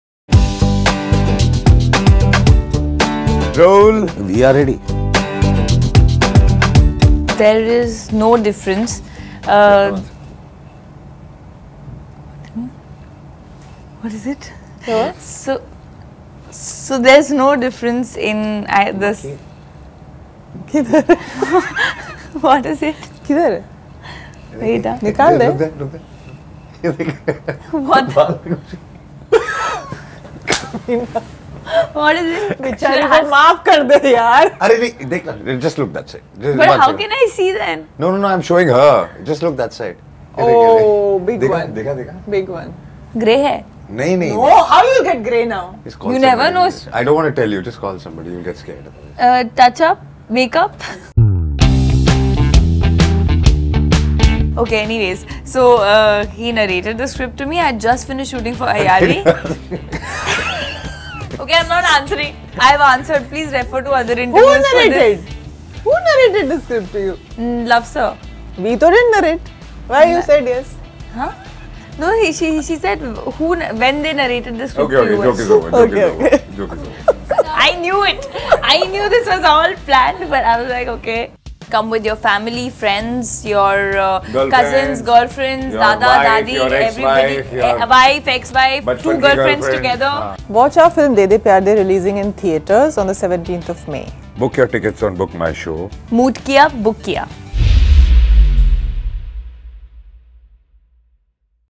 PRANK Moment With Ajay Devgn, Tabu and Rakul Preet | De De Pyaar De movie
Watch the video as Bollywood's popular prankster Ajay Devgn teams up with Tabu to pull a prank on co-star Rakul Preet Singh while promoting their upcoming movie De De Pyaar De.

De De Pyar De releases on 17th May 2019 and you can book your tickets only on BookMyShow